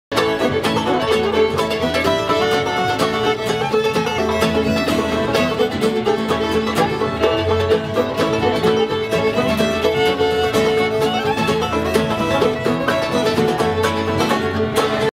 South End Hops Fest Charlotte NC
It's always lively at the South End Hops Fest hosted by Elevate Lifestyle.